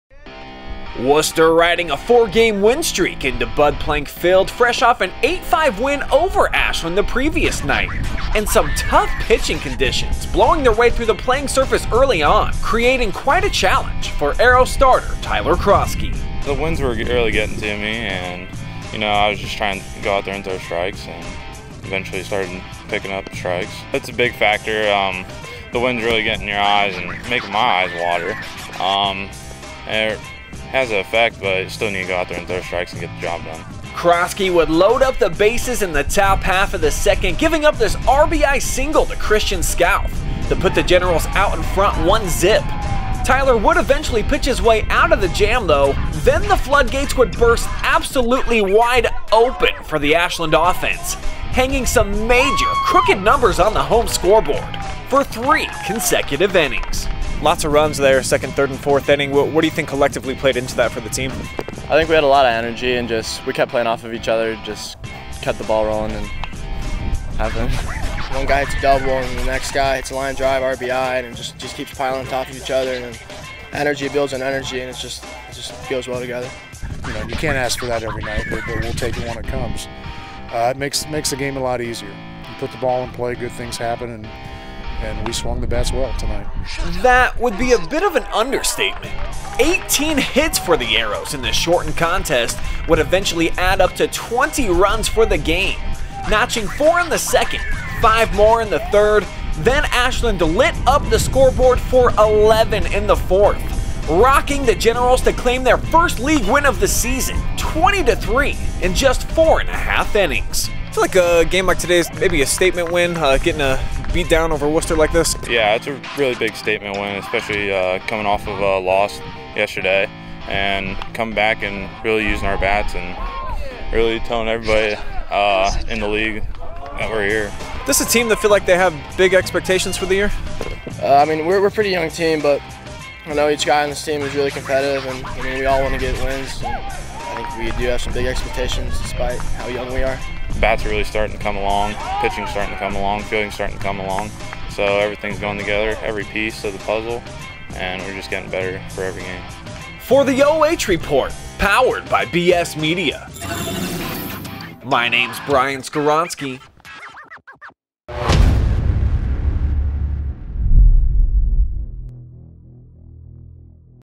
Ashland Rocks Wooster 20-3 Behind 18 Hits
Ashland baseball coach Rick Gough was anxious to get his team back on the field after dropping an 8-5 contest at Wooster on Tuesday night, feeling his squad could perform better. The Arrows skipper would prove himself right the following afternoon, knocking out a season-best 18 hits in a 20-3 beatdown of the Generals, who had won their last 4 games, in a shortened 4 ½ innings.

Right-hander Tyler Croskey picked up the first OCC victory for Ashland, giving up 3 runs on 3 hits while walking 4 and striking out 4 Generals in 4 innings of work. A stiff and steady breeze would get to Croskey early in the contest, walking the bases loaded in the top half of the 2nd leading to an RBI single from Kristian Scalf and a 1-0 Wooster advantage.

The floodgates would then be opened for the Arrows offensive attack, chalking up 4 runs in their half of the 2nd, then cranked out 5 runs on 4 hits in the 3rd before lighting up the home scoreboard for 11 4th inning runs to put the game out of reach.

Arrows sophomore first baseman Cole Garrison had a monster night from the dish, going 2-4 with a double, 5 RBI and scored 3 times. Second baseman Drew Schaly went 3-5 with a double, 3 RBI and a run while shortstop Brennen Hunt finished 1-3 from the plate with 3 RBI.

Ashland (2-1, 1-1) will next play at Norwalk on Saturday while Wooster (5-4, 4-1) hosts rival Orrville on Thursday.